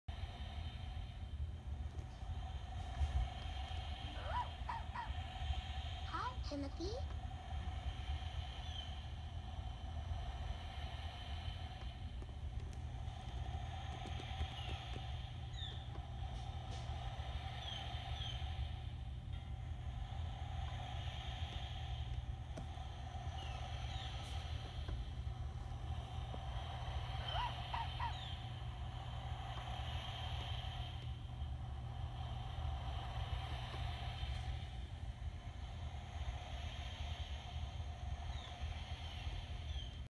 Timothy's new crib
I even re-did the new animal baby, Tiffany's cradle. The ocean mobile was a little too heavy for the cradle to rock. So I used one of the crib soothers I have.